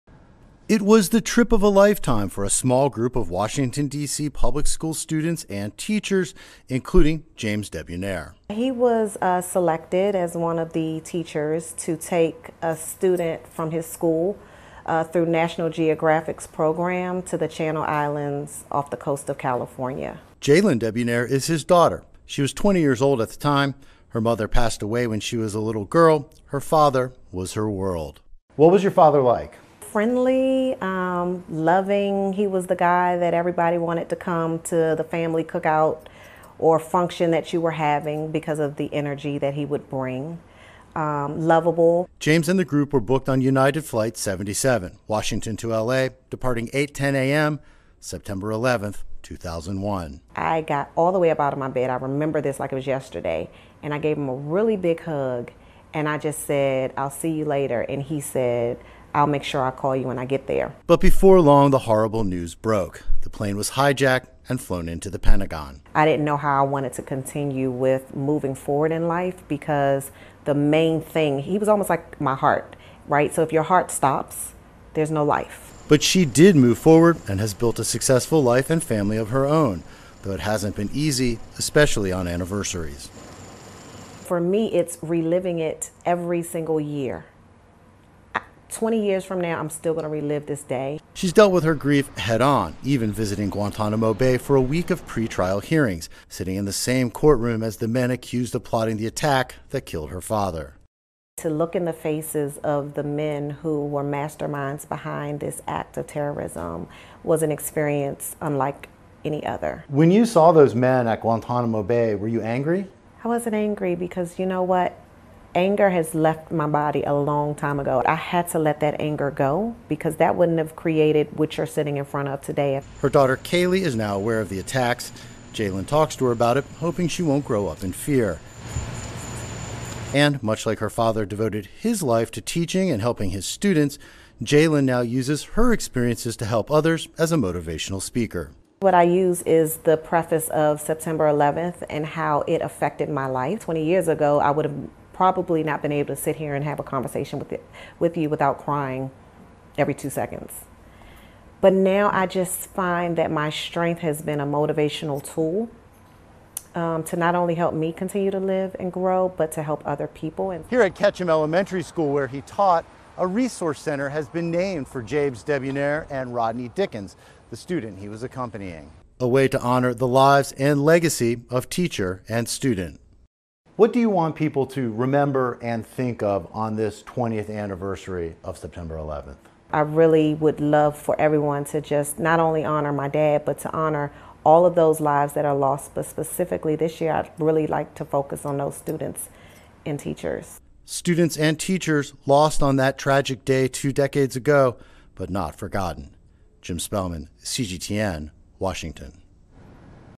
SEPTEMBER 11 TWENTY YEARS ON: Daughter remembers father killed in terror attack
Nearly three thousand people died in the terrorist attacks of September 11th, 2001. Among them, three teachers and three students from Washington, DC. The daughter of one of those teachers is sharing memories of her father and the impact that terrible day has had on her life. Jim Spellman has more.